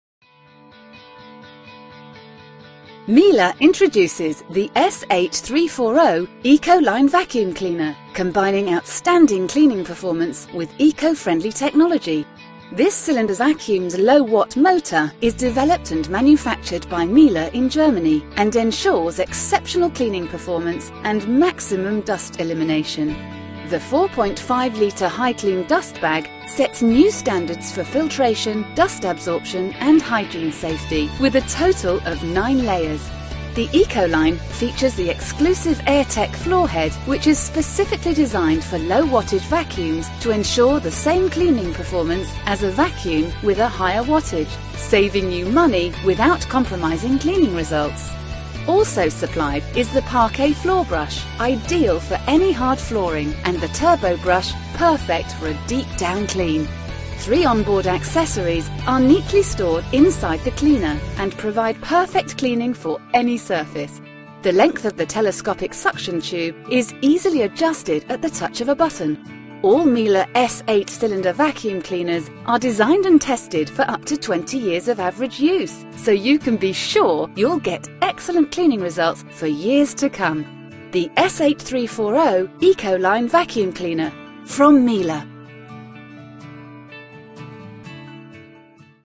Miele S8340 Ecoline S8 Series Cylinder Vacuum Cleaner
The Miele S8340 Ecoline Vacuum cleaner combining outstanding cleaning performance with eco friendly technology this cylinder vacuum's low watt motor is developed and manufactured by Miele in Germany and ensures exceptional cleaning performance and maximum dust elimination the 4.5 litre high clean dustbag sets new standards for filtration dust absorption and hygiene safety with a total of nine layers. This impressive Miele Vacuum Cleaner is available in stock at Hispek.  The Ecoline features the exclusive airtech floorhead which is specifically designed for low wattage vacuums to ensure the same cleaning performance as a vacuum with a higher wattage saving you money without comprising cleaning results also supplied is the parquet floor brush ideal for any hard floorings and the turbo brush perfect for a deep down clean. Three on board accessories are neatly stored inside the cleaner and provide perfect cleaning for any surface the length of the telescopic suction tube is easily adjusted at the touch of a button.  The Miele S8340 Ecoline Vacuum cleaner is on demonstration at Hispek Superstore in St Albans. All Miele S8 cylinder vacuum cleaners are designed and tested for up to 20 years of average use so you can be sure you will get excellent cleaning results for years to come. The S8340 Ecoline vacuum cleaner from Miele.

Miele S8340 Ecoline S8 Series Cylinder Cleaner has a stylish Ivory White finish. Miele S8340 delivers outstanding cleaning performance and exceptional cleaning results. The S8340 comes with 2 year warranty for extra peace of mind. Available to buy from Hispek.